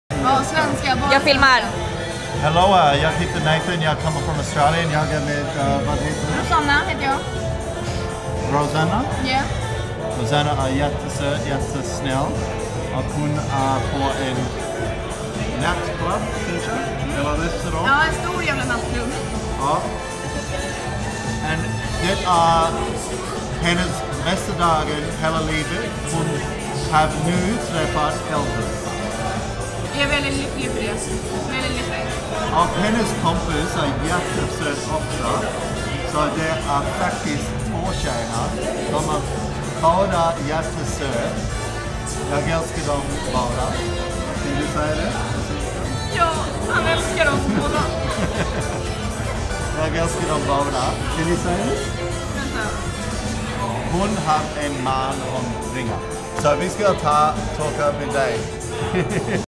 Elvis Nathan speaks broken Swedish with Rosanna
Elvis Nathan shows us his interesting Swedish skills until the phone rings. In Sweden it is extremely common for the women to consistently text and phone. This is one of the biggest crazes in Sweden.